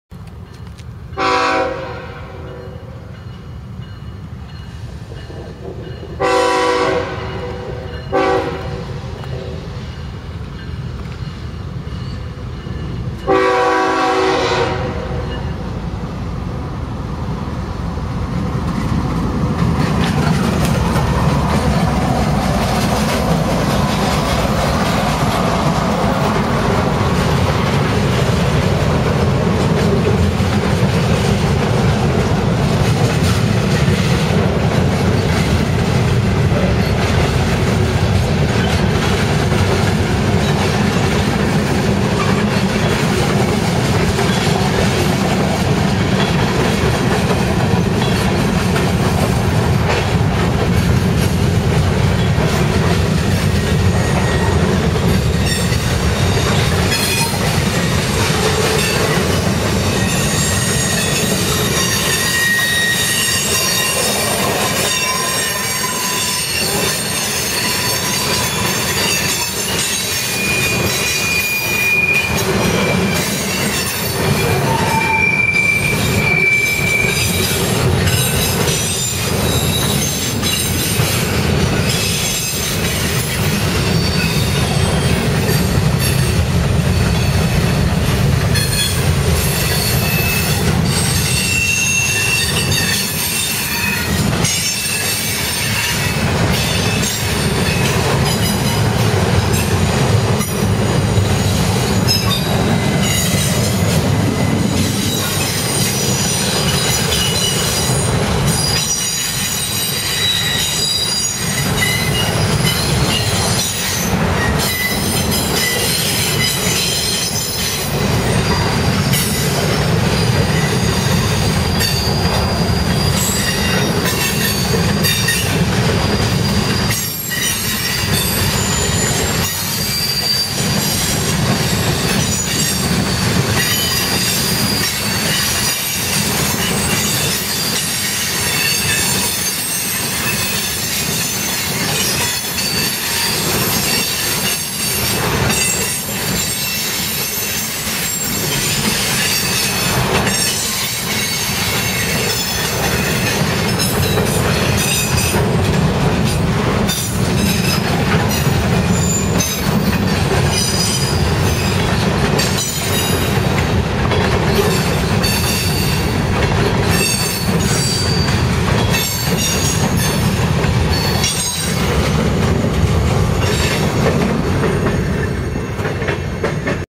Canadian Pacific Railway #243. Elkhart Indiana August 7th, 2013
Westbound Canadian Pacific run-through freight #243 is arriving at Elkhart Indiana on Norfolk Southern rails for a crew change before continuing west for Bensenville Illinois.